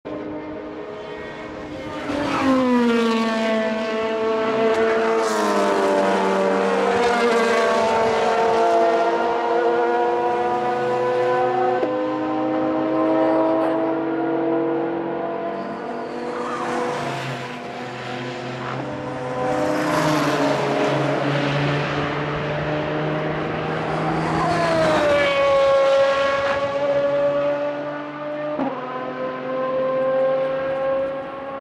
KartingCurvesAudiInChargeLateNight
With the Peugeots having overheating and mechanical problems, the Audis pushed hard all night.
Maybe the best spot to watch the race for us, back close to our tent site. We were again on top of the Karting pits, overlooking the fence. not many people there, either.